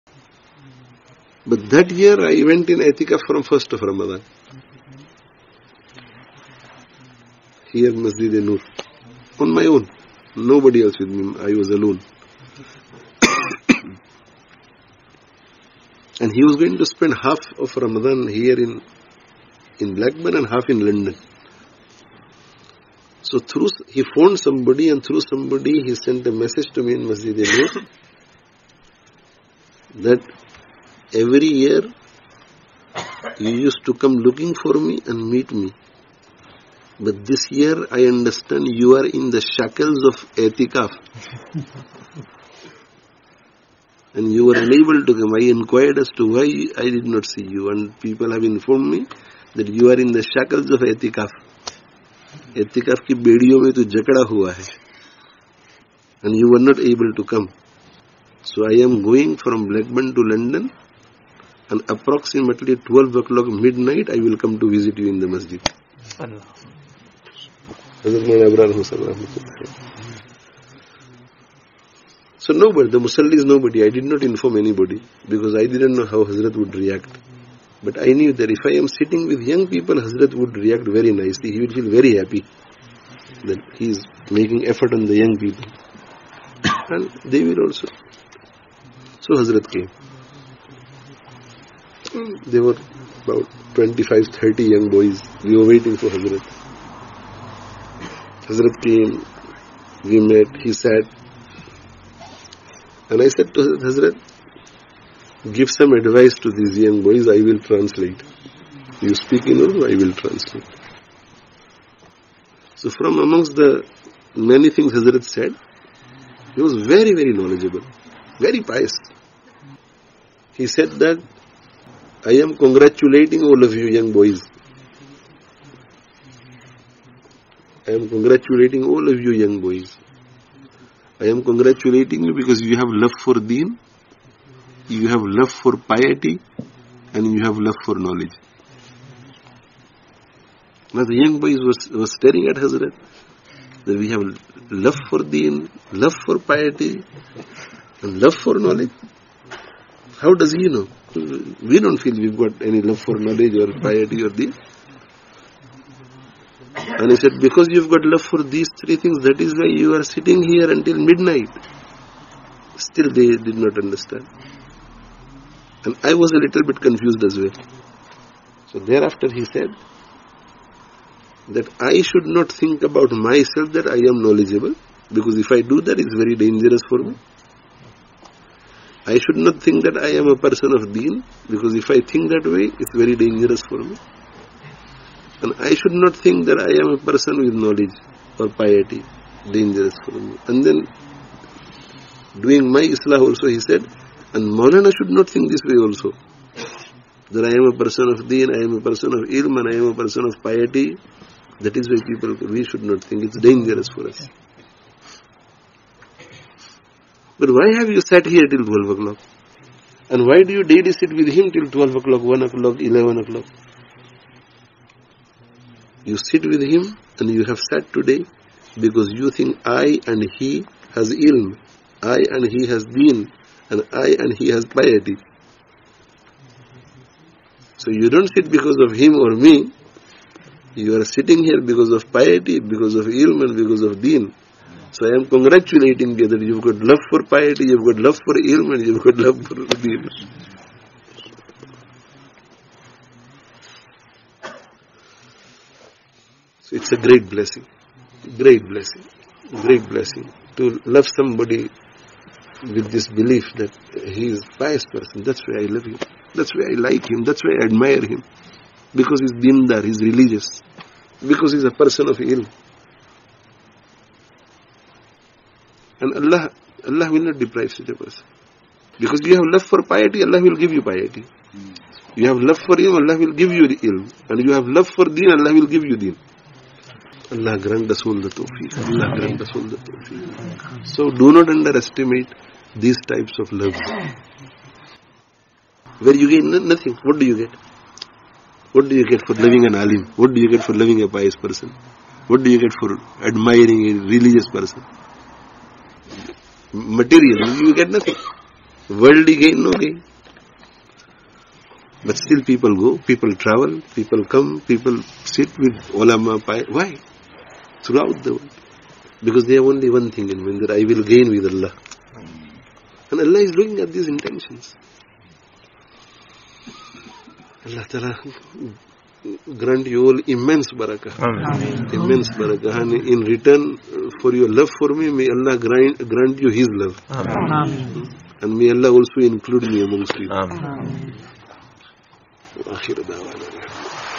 In Pious Company-An incident with Hadrat Mawlana Abraar Ahmed Dhulyawi RA | Shaykh Saleem Dhorat DB
Listen to an incident which occurred in the early 90's when the respected Shaykh hafizahullah was honoured by the visit of Hadrat Mawlana Abraar Ahmed Dhulyawi rahimahullah, in the Month of Ramadhan, during itikaaf, in Masjid an Noor Leicester.
 'Yaadgaar Lamhe-Memorable Moments'